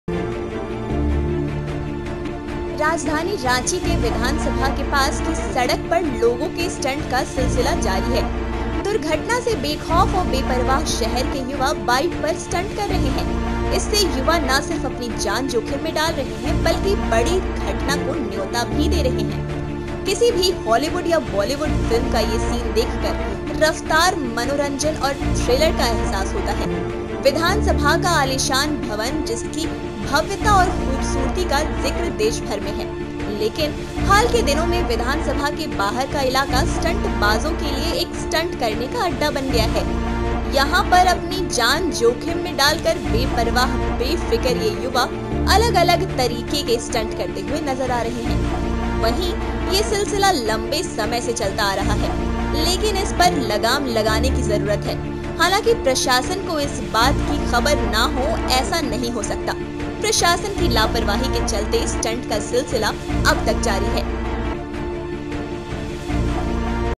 Ranchi में युवा कर रहे जानलेवा 'स्टंट', जोखिम में डाल रहे अपनी और दूसरों की जान | वनइंडिया हिंदी
People's stunts continue on the road near the assembly of the capital Ranchi. Unhappy and unaware of the accident, the youth of the city are doing stunts on bikes. With this, the youth are not only putting their lives at risk but are also inviting the big event. Seeing this scene from any Hollywood or Bollywood movie, one feels a sense of speed, entertainment and thrill. The majestic building of the assembly, whose grandeur and beauty is mentioned throughout the country, but in recent times, the area outside the assembly has become a stunt base for stunters.

राजधानी रांची के विधानसभा के पास की सड़क पर लोगों के स्टंट का सिलसिला जारी है. दुर्घटना से बेखौफ और बेपरवाह शहर के युवा बाइक पर स्टंट कर रहे है. इससे युवा ना सिर्फ अपनी जान जोखिम में डाल रहे हैं बल्कि बड़ी घटना को न्यौता भी दे रहे हैं. किसी भी हॉलीवुड या बॉलीवुड फिल्म का ये सीन देखकर रफ्तार, मनोरंजन और थ्रिल का एहसास होता है. विधानसभा का आलीशान भवन जिसकी भव्यता और खूबसूरती का जिक्र देशभर में है.लेकिन, हाल के दिनों में विधानसभा के बाहर का इलाका स्टंटबाज़ों के लिए एक स्टंट करने का अड्डा बना है.